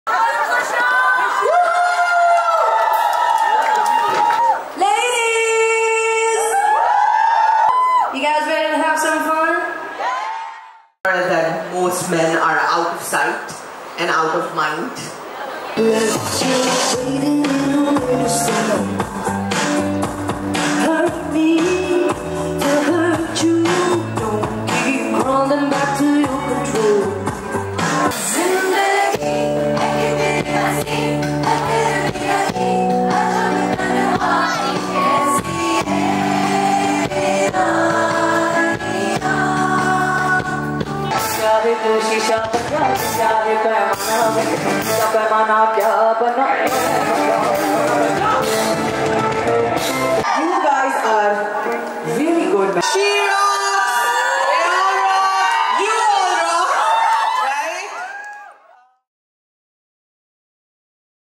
Teaser- She Rockx I 17th March 2023 I Natasha Baig, Shae Gill & Maria Unera
Creative Headquarters, a newly launched production house and event management company, is already making history by organizing the first-ever all-femme concert, SHE ROCKX. The concert showcases the exceptional talents of Shae Gill, Natasha Baig, and Maria Unera and demonstrates CHQ’s director Vinessa Phoebe’s strong commitment to empowering women. The concert was exclusively for women, providing a joyful evening of dancing, singing along to the music, and savoring delicious food. Creative Headquarters aims to promote gender equality and inclusivity through a range of production services and exciting events, including women empowerment campaigns.
Location: District19

Artists:
Maria Unera
Shae Gill
Natasha Baig

Host:
Khushu Naheed